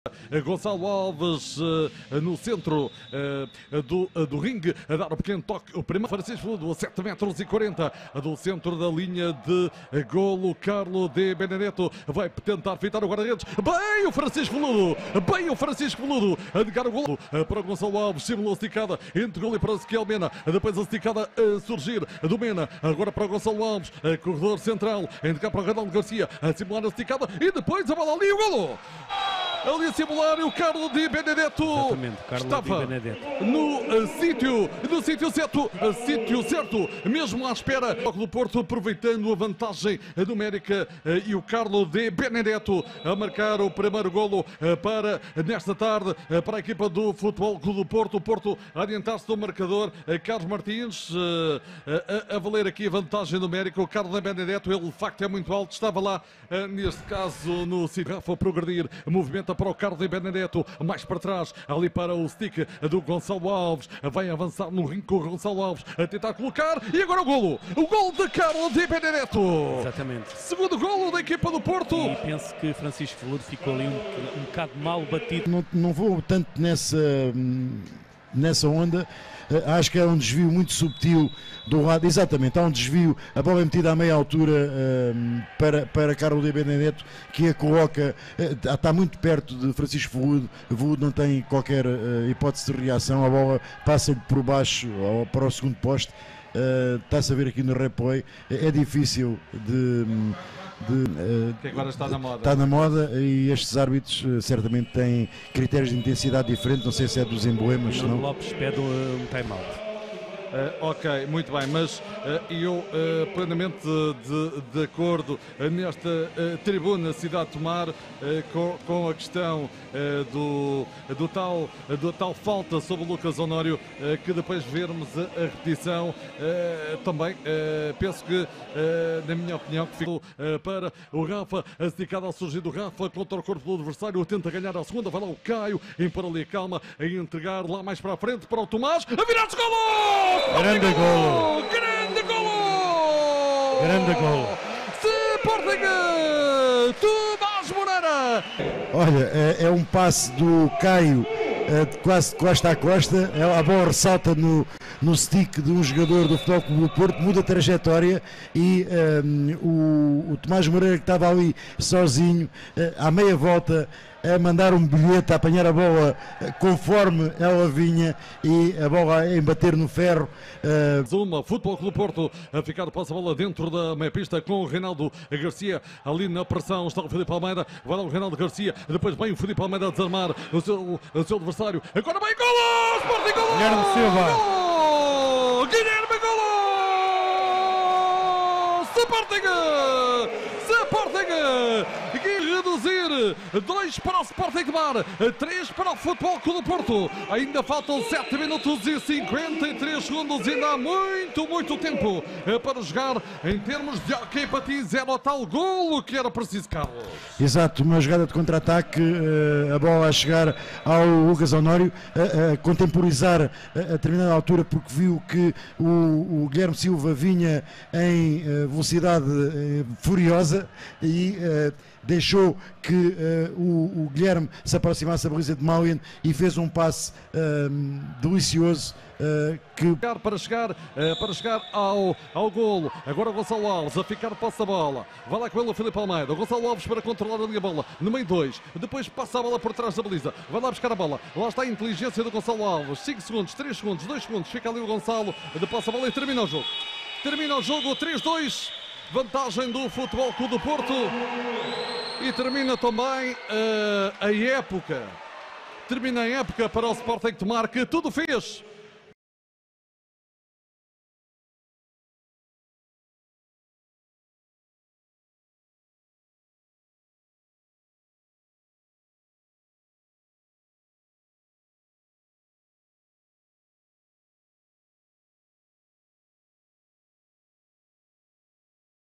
Highlights SC Tomar vs FC Porto (1/4 final play off CN 1ª DIVISÃO)
CAMPEONATO NACIONAL 1ª DIVISÃO - PLAY OFF - 1/4 DE FINAL - 2ª JORNADA (Imatges de la retransmissió de Rádio Cidade de Tomar).

HIGHLIGHTS:

00:00 Inici
00:17 0-1 Carlo Di Benedetto
00:58 0-2 Carlo Di Benedetto
01:41 0-3 Gonçalo Alves
02:10 1-3 Tomás Moreira
02:51 2-3 Guilherme Silva